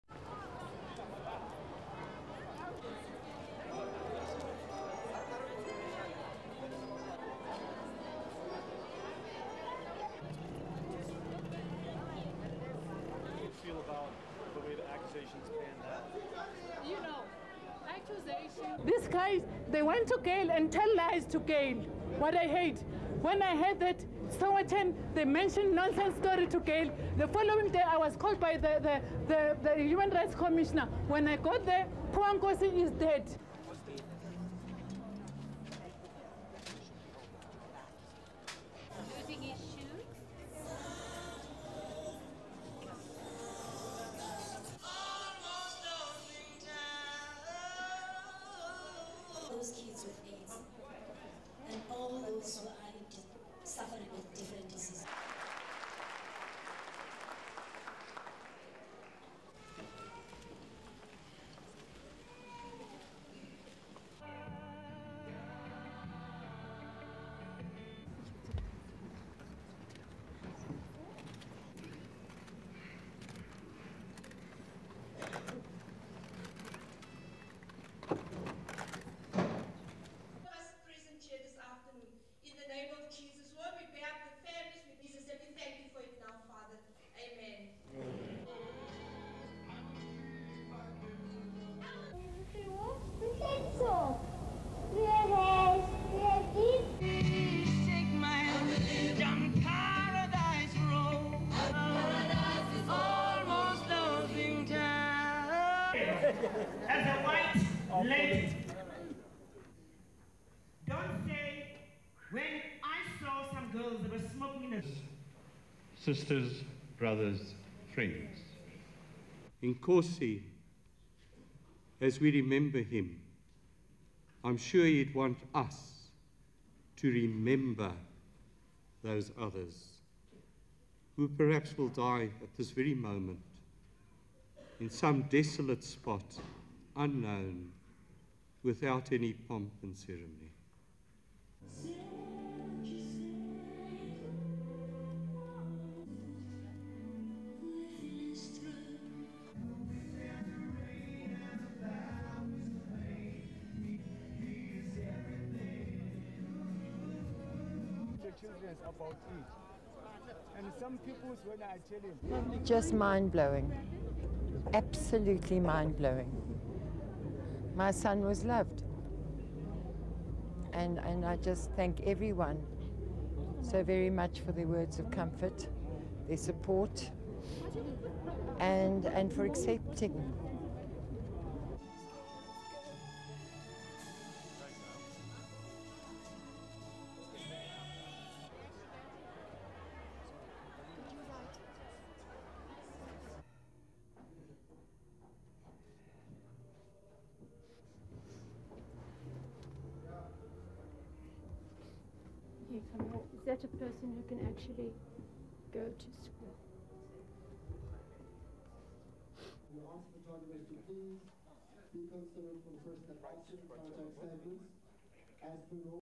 Memorial for AIDS activist Nkosi Johnson
APTN - June 6, 2001 - Johannesburg
1. Exterior Central Methodist Church
2. Various arrivals
3. Various of Aids activists handing out stickers
4. Set up Hilda Khoza
5. SOUNDBITE (English) Hilda Khoza, reflexologist
"These guys, they went to Gail and told lies to Gail. What I hate, they mentioned nonsense story to Gail. The following day I was called by the U-N rights commissioner. When I got there, poor Nkosi is dead."
6. Gail Johnson and daughter Nicky arriving
7. Still of Nkosi
8. Various interior church
9. Still of Nkosi Johnson
10. Gail Johnson and Nkosi's biological grandmother Ruth Khumalo lighting candles

POOL - Durban - July 2000
11. Nkosi addressing Aids conference

12. Choir singing hymn
13. SOUNDBITE (English) Reverend Bryan Oosthuizen in English
"Sisters, brothers, friends. Nkosi, as we remember him, I'm sure he'd want us to remember those others who perhaps will die at this very moment in some desolate spot, unknown and without any pomp and ceremony."
14. Various of Gail Johnson and children lighting candles
15. SOUNDBITE: (English) Gail Johnson
"Just mind-blowing, absolutely mind-blowing. My son was loved and I just thank everyone so very much for their words of comfort and support and for accepting."
16. Mid shot of Gail Johnson crying

APTN - 31 May 2001 - Johannesburg
17. Johnson at Nkosi's bedside
18. Various of emaciated Nkosi lying in bed

APTN - Durban
19. Nkosi seated for presser

STORYLINE:

Hundreds of mourners packed a church in downtown Johannesburg on Wednesday for a memorial service for Nkosi Johnson.

The 12-year-old South African Aids activist died of the disease on Friday.

His foster mother, Gail Johnson, who cared for him since he was two years old, was joined by friends, family and relative to celebrate a short life and mourn its passing.

School children joined celebrities and Aids activists in paying tribute to South Africa's longest surviving child with the disease.

Among the mourners was Johannesburg reflexologist Hilda Khoza.

A day before Nkosi's death, she had reportedly accused Gail Johnson of abusing the boy for money and not letting him go to school.

Speaking to APTN, she angrily denied making the claims.

Nkosi Johnson made international news last year when he addressed thousands of delegates at the 13th International AIDS Conference in Durban and asked that AZT be given to pregnant HIV-positive women.

Then in December, Nkosi collapsed  with Aids-related brain damage and viral infections.

He became bedridden and in a semi-comatose state as his body wasted away.

Nkosi died at home last Friday.